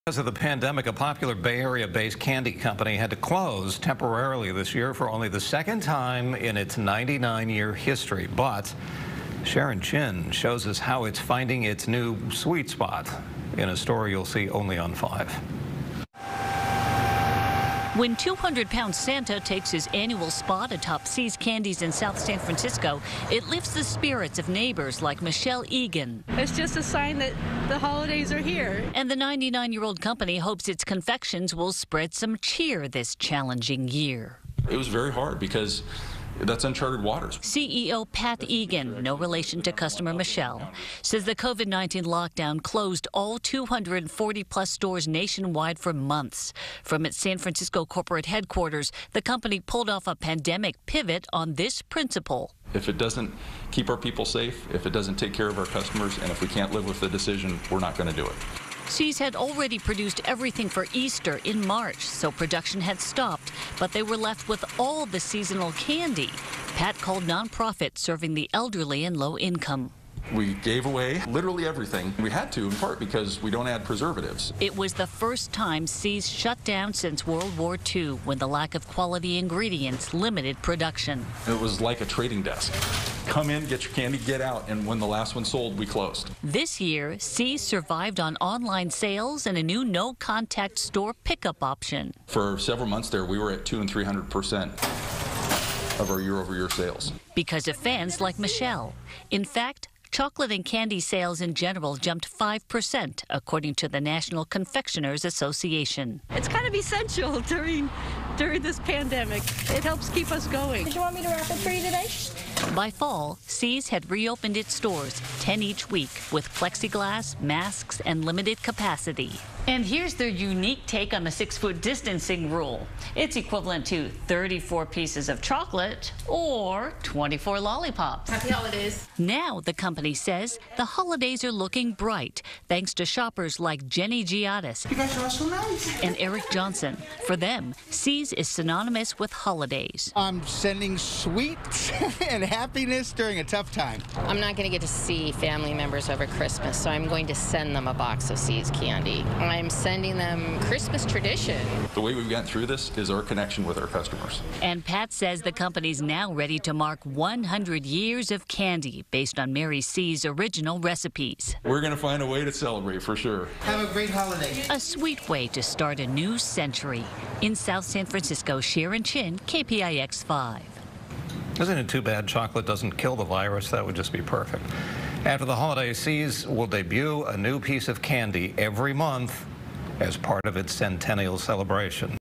Despite Pandemic, Sees Candy Prepares to Celebrate a Sweet Century
Sees Candy had to close temporarily for only the second time in its 99-year history because of the COVID-19 pandemic but now Sharon Chin shows how it's finding a new sweet spot. (12-24-20)